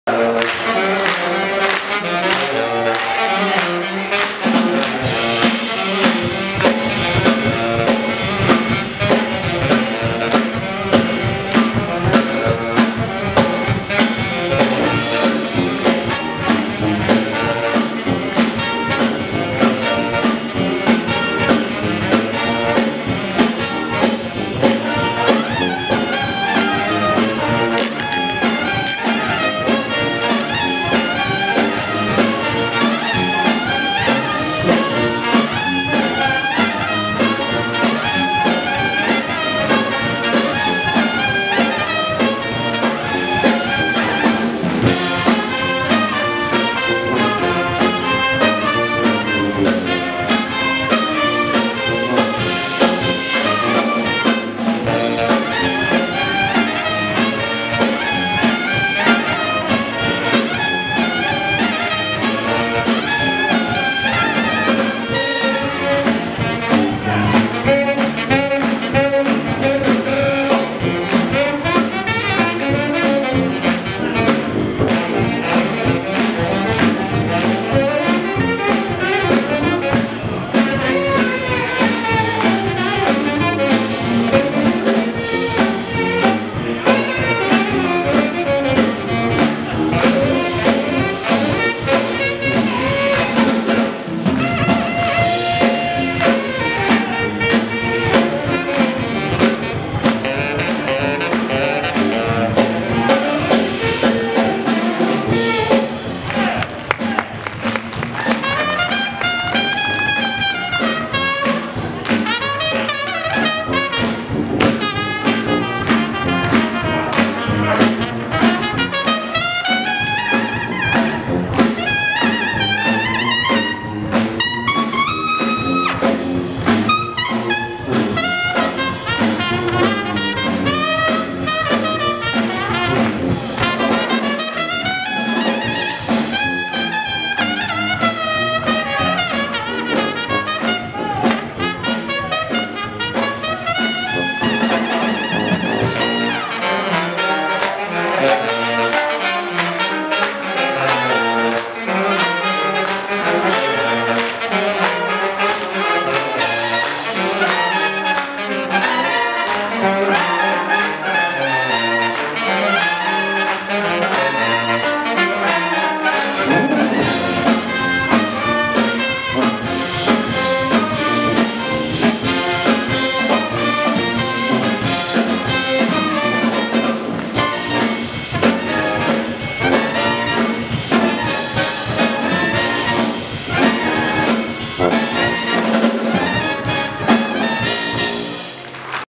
Ian Cooper with USN 7th Fleet Broadside Brass Band
Great to watch Ian Cooper play during our visit to Japan. The Japanese love jazz but are much more reserved in their applause and participation.